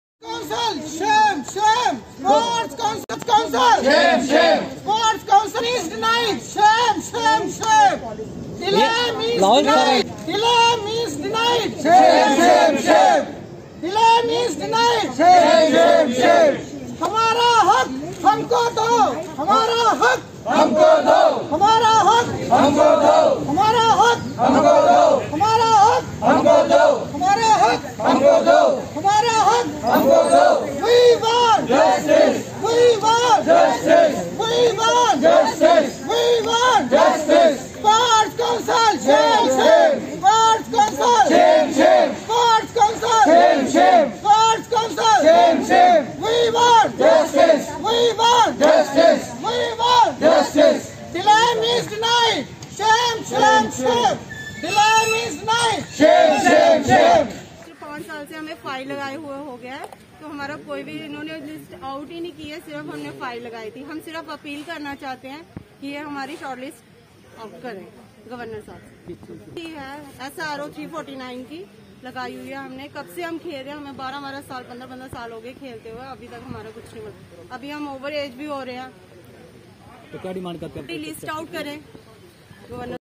Protest by sports persons on delaying in framing Sports Policy and not Providing job
Protest by sports persons on delaying in framing Sports Policy and not Providing job to meritorious sports persons